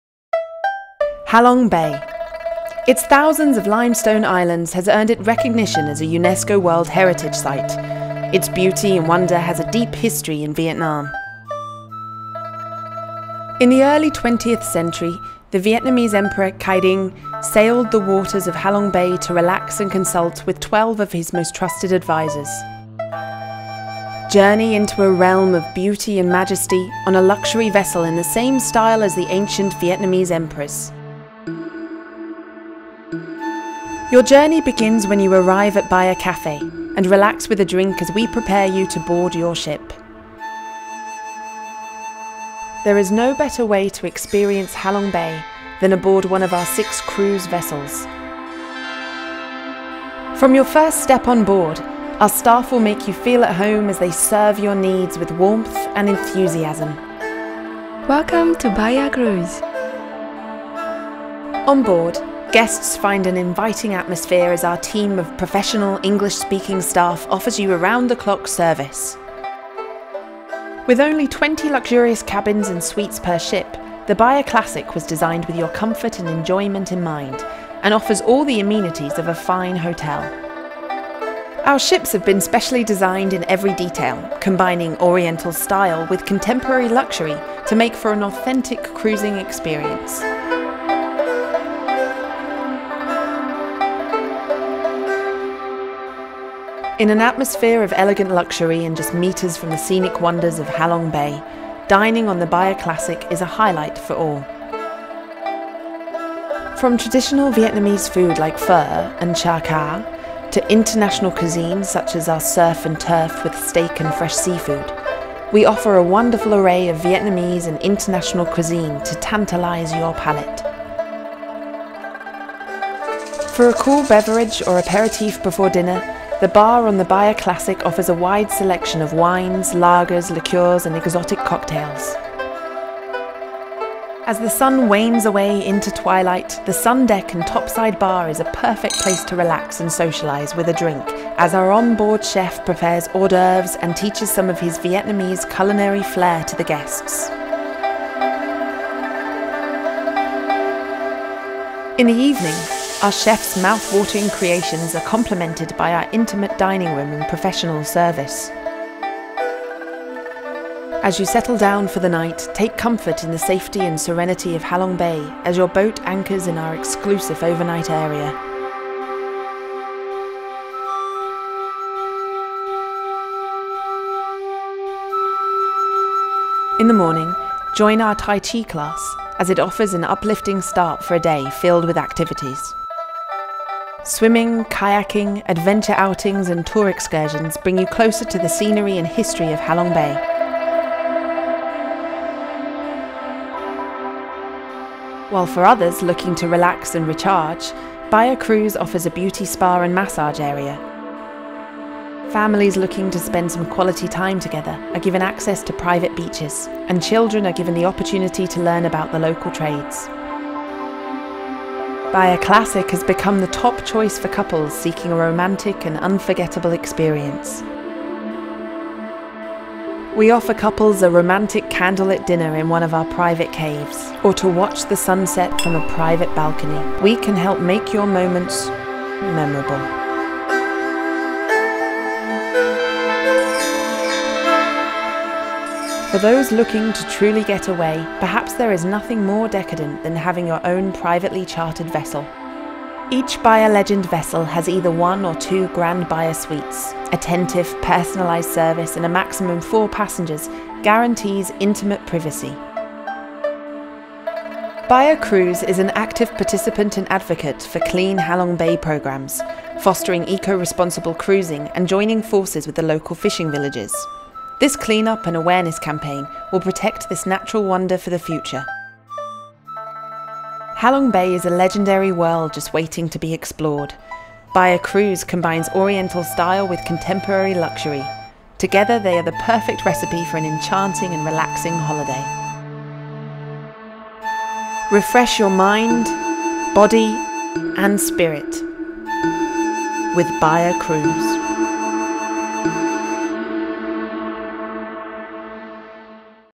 Bhaya Cruises: Luxury cruises in Halong Bay, Vietnam - HD
Discover Halong Bay and Cat ba Island- Experience Bhaya
Discover the breathtaking scenery of Vietnams premier destination aboard a cruise vessel which exudes charm and luxury!